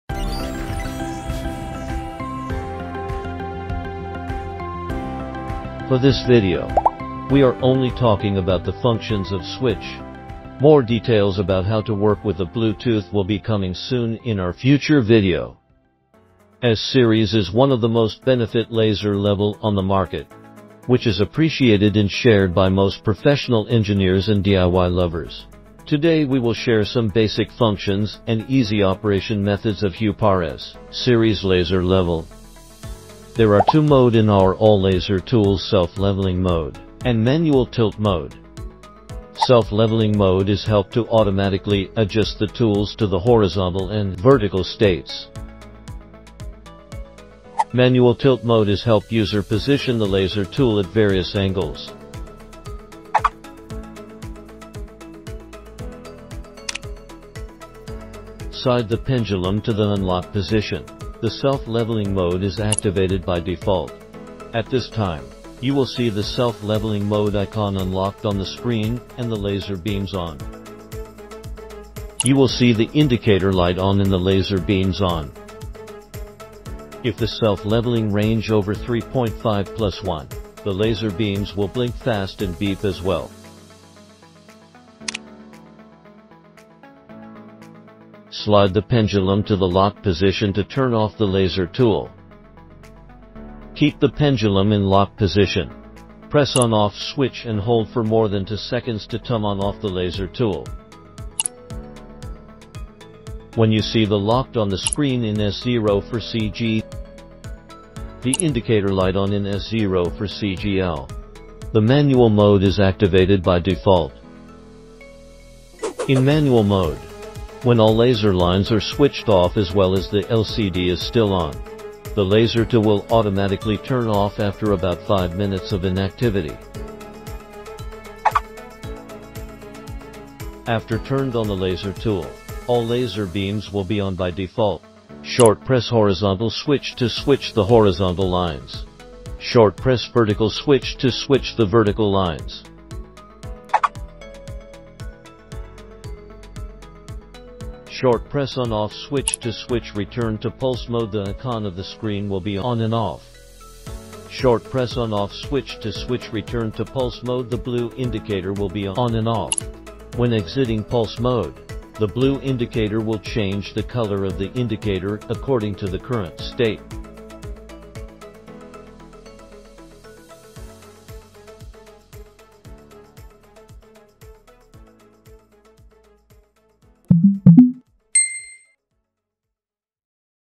How to use the Self-leveling Mode and Manual/Tilt Mode of Huepar S04CG/S04CG-L laser level
In this video, we shared some basic operations about our high-end model-S04CG and other S series for you.
Huepar Lase Level is almost the only tool on the market that incorporates Bluetooth technology. For example, connect S04CG Laser Level with Free Huepar APP via Bluetooth Technology, users can control the laser tool by Mobile phone. The laser tool can connect two phones at the same time, whether on Android or IOS.

We will have a video soon, to talk about how to work with Bluetooth and our exclusive APP.

Huepar S04CG Self-Leveling 4*360-Degree Cross-Line Laser projects a bright, continuous, horizontal line around an entire room, up to 200 ft. in diameter, making it easy to hang pictures at the same precise height, install chair rail molding or a wallpaper accent, and more. In addition to the 360° continuous level line, it can project plumb cross lines to accurately guide a range of DIY projects. The laser level kit features an LCD screen display. Users can easily get the status of the power, angle, pendulum, and pulse mode on the display.